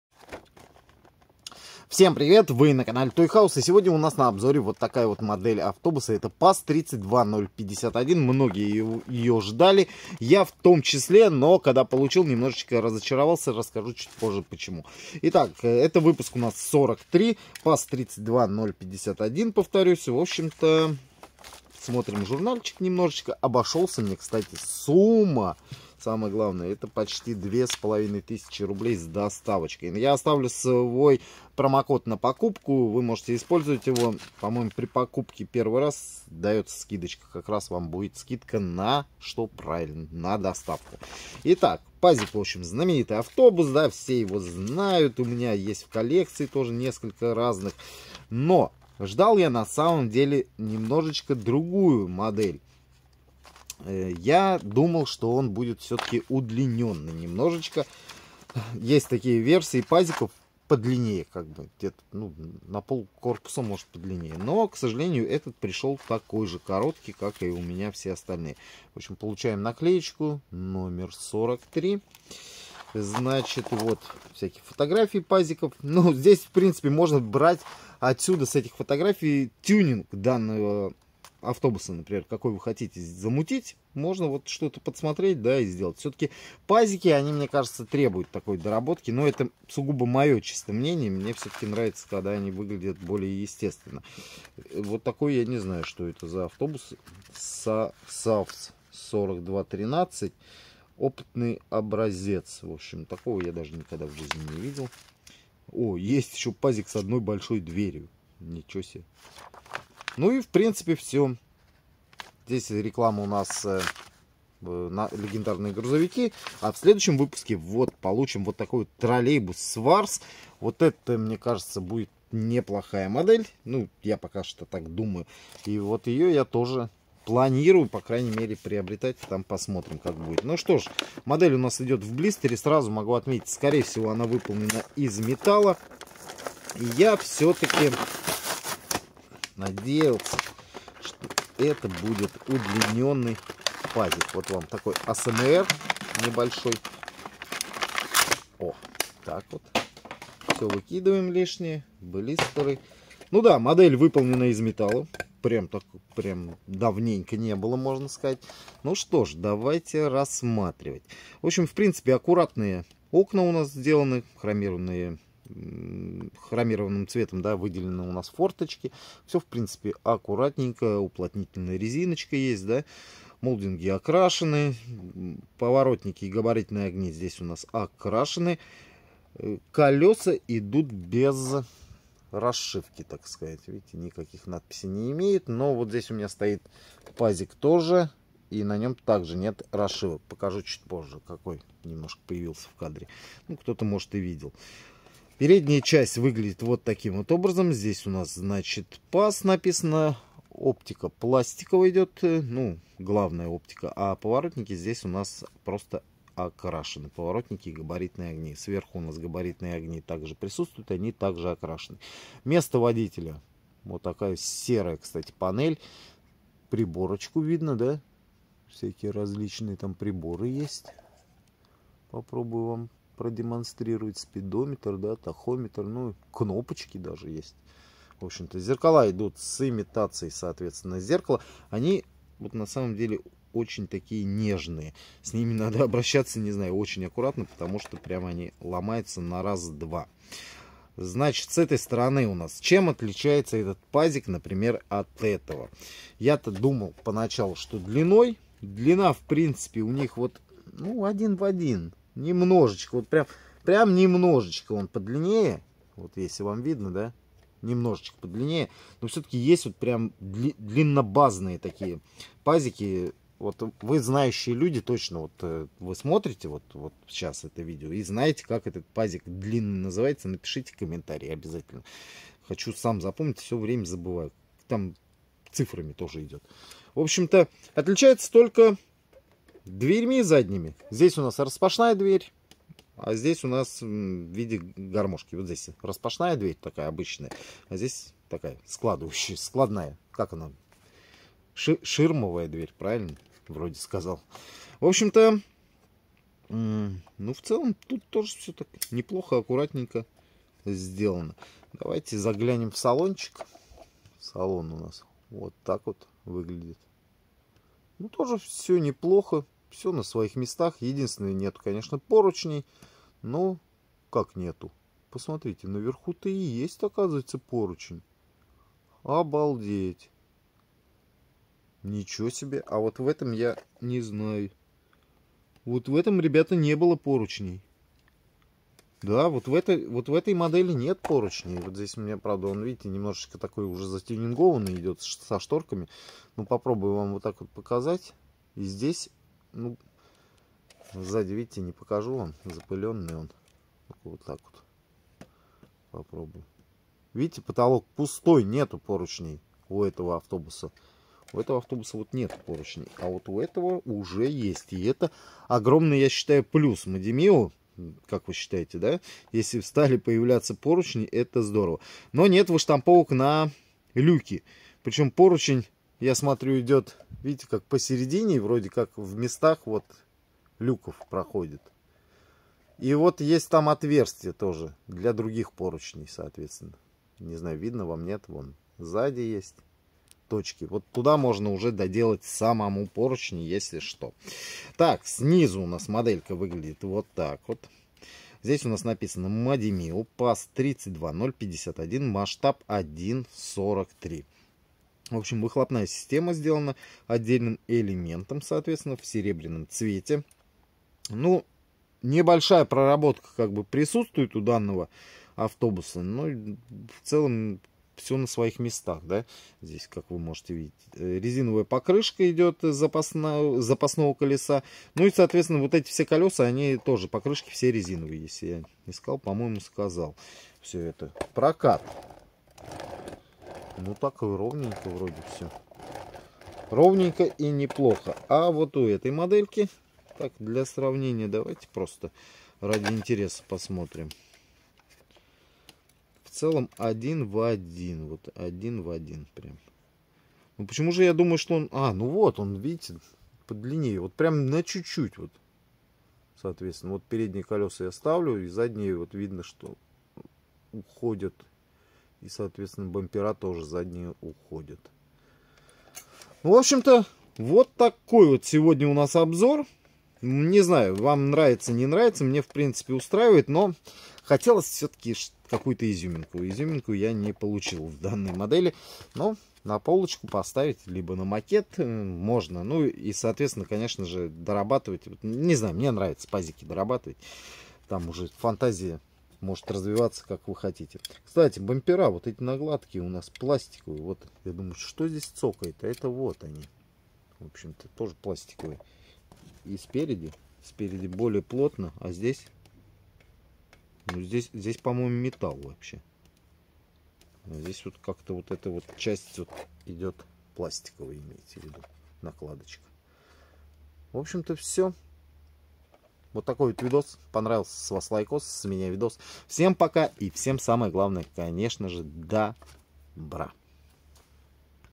Модель АВТОБУС ПАЗИК масштаб 1/43. Наши Автобусы №43. Про машинки.
Масштабная модель автобуса ПАЗ 32051 от компании Модимио журнальная серия Наши Автобусы выпуск №43. Распаковка и обзор металлической коллекционной модели. Видео про машинки модельки.